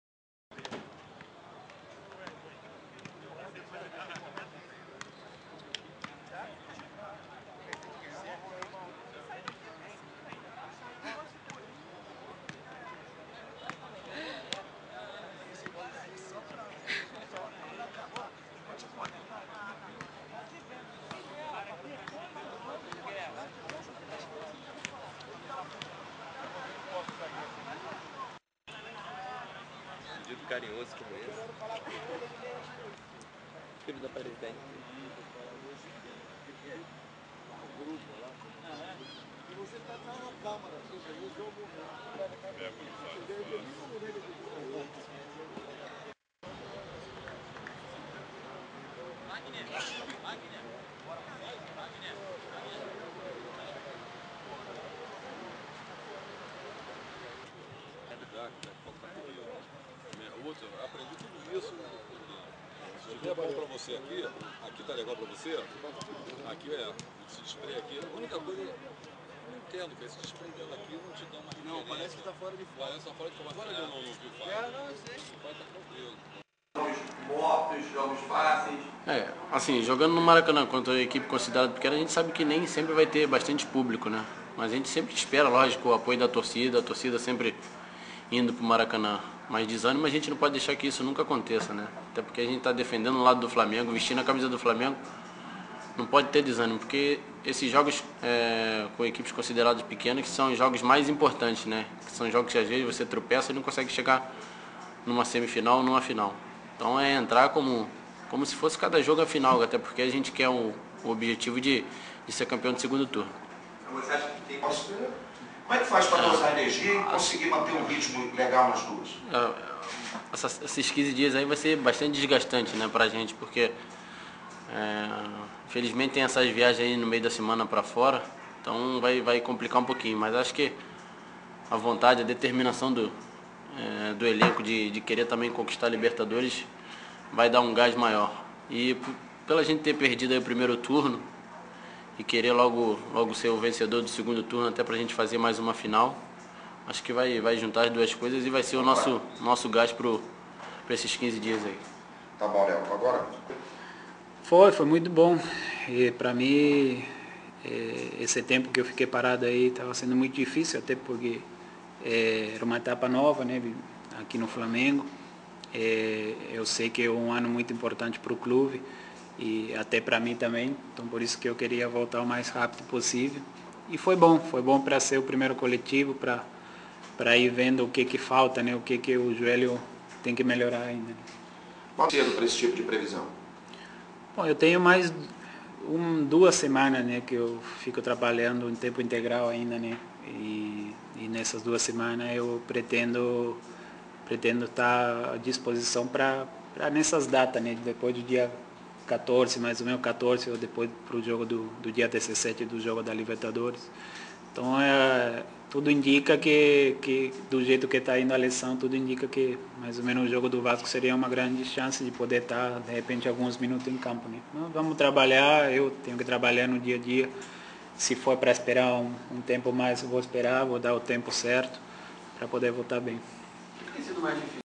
Treino do Flamengo na Gávea dia 04/03/2010
O time rubro-negro voltou a treinar na Gávea, após a vitória por 2 a 0 sobre o Madureira, no Maracanã. Durante a coletiva de imprensa o meia Maldonado comentou sobre seu primeiro coletivo e acredita estar a disposição do técnico Andrade na partida contra o Vasco, pela Taça Rio.
Já o lateral, Léo Moura, falou que a equipe tem que ter atenção com os times considerados pequenos e comentou sobre o desgaste que o grupo vai ter durante as viagens para os jogos da Libertadores.